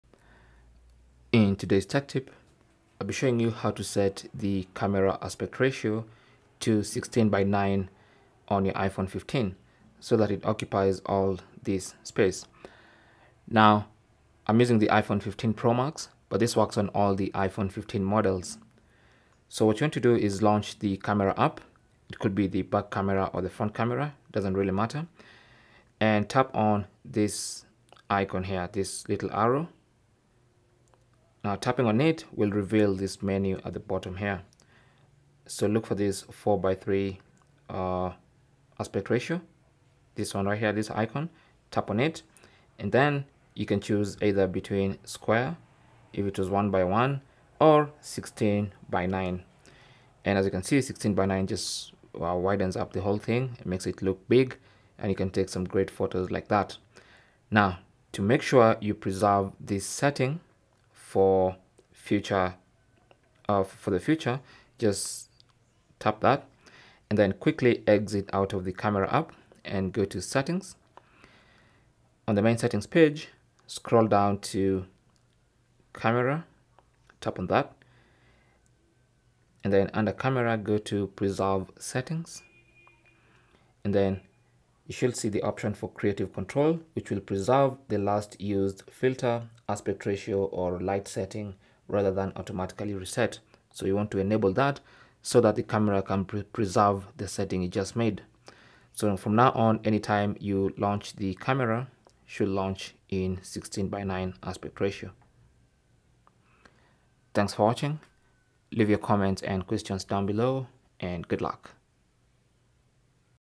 How To Set Camera Aspect Ratio To 16:9 On iPhone 15 & iPhone 15 Pro
Learn how to set the camera aspect ratio to  16:9 on iPhone 15 and iPhone 15 Pro.

Aspect ratio refers to the proportional relationship between the width and height of an image or video frame. Different aspect ratios can be used to achieve specific visual effects or accommodate various display formats.

The 16:9 aspect ratio is a popular choice for many because it is widely supported on various devices and is commonly used in video production, making it great for capturing cinematic shots.

In this tutorial video, we'll show you how to set the camera aspect ratio to 16:9 on your iPhone 15 or iPhone 15 Pro, giving you more control over your photos and videos.

How To Set Camera Aspect Ratio To 16:9 On iPhone 15
Launch Camera
Tap on the arrow at the top of the screen
Tap on 4:3
Choose 16:9

Samsung Galaxy Z Flip 5:

Samsung Galaxy Watch 6/ 6 Classic: (Customize Your Order):

Cell Phone Tripod Adapter